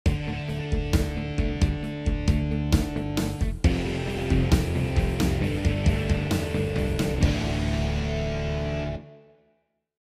Rock and Olek channel intro - 8 years old guitarist - Olek Brzezinski
Aleksander "Olek" is an 8 year old guitarist.
He is learning to play the electric guitar.
Olek likes playing on the guitar covers of popular songs.
He will be happy if you will like it :)
Keep watching Rock and Olek channel!